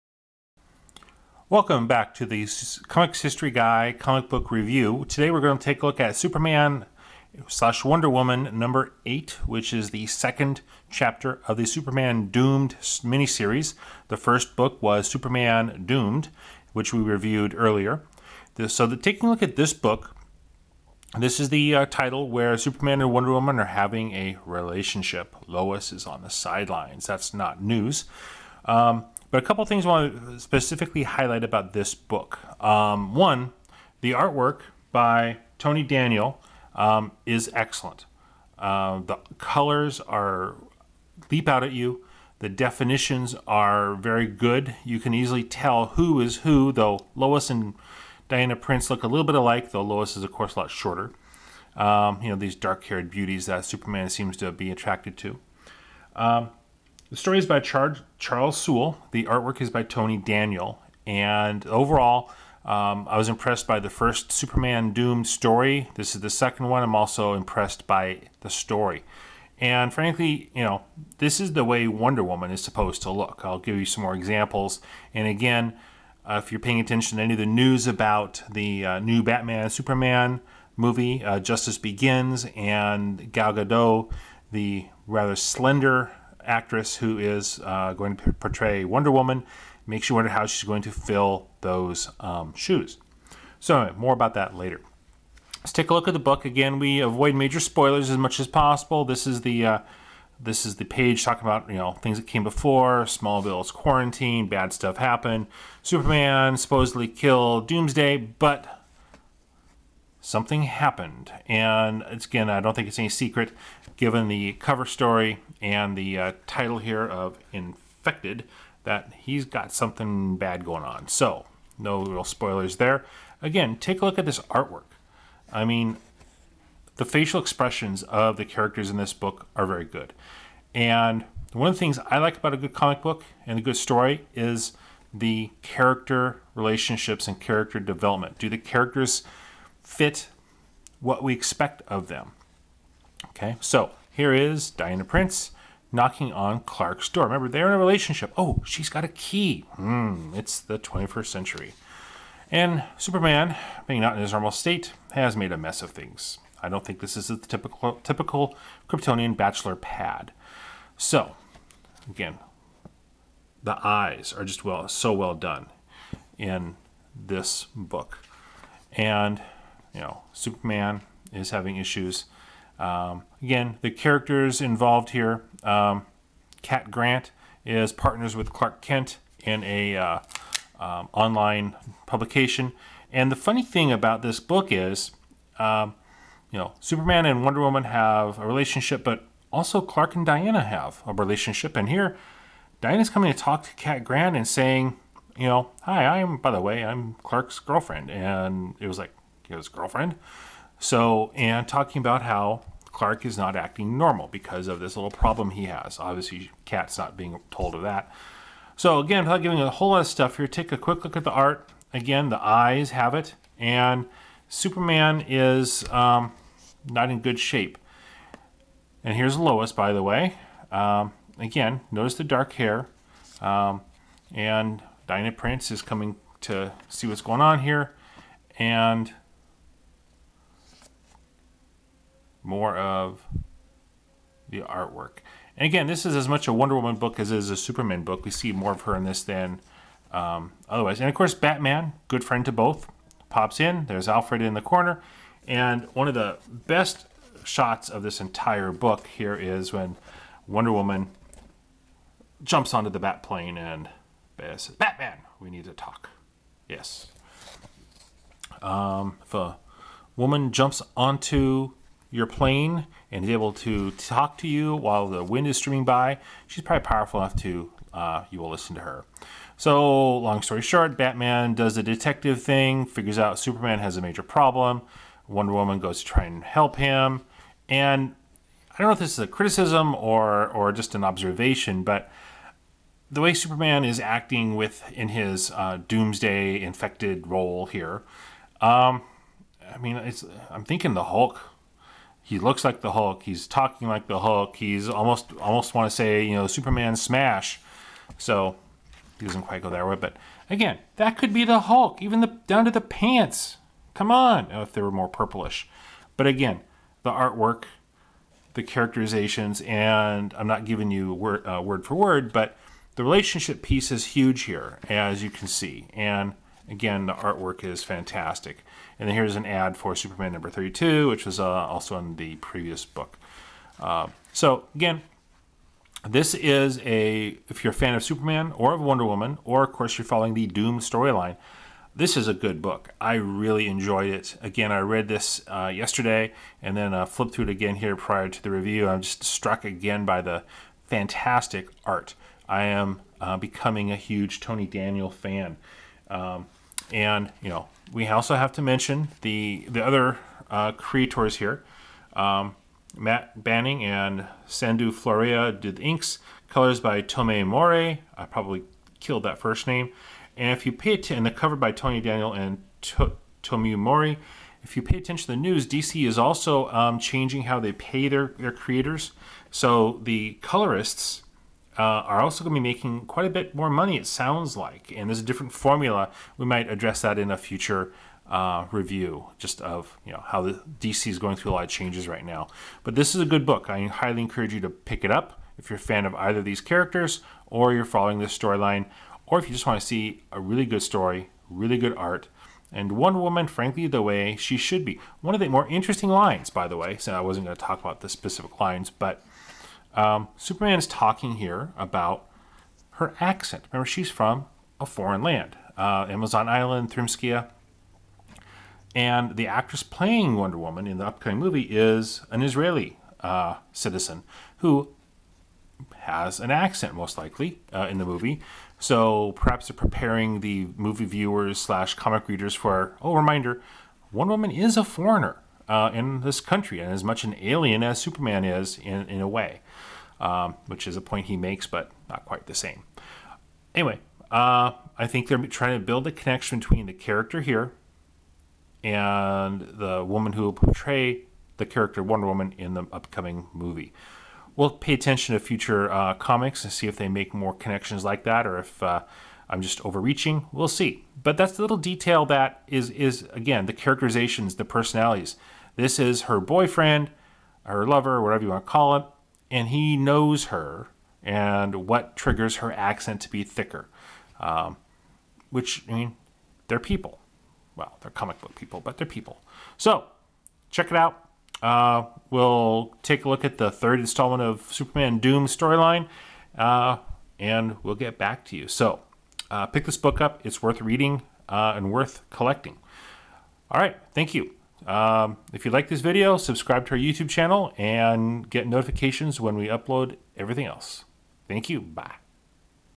Superman/Wonder Woman #8 Comic Book Review-for Superheroreviews.com
Review of the DC Comics' New 52 Superman/Wonder Woman #8.  This is the second chapter of the Superman: Doomed storyline.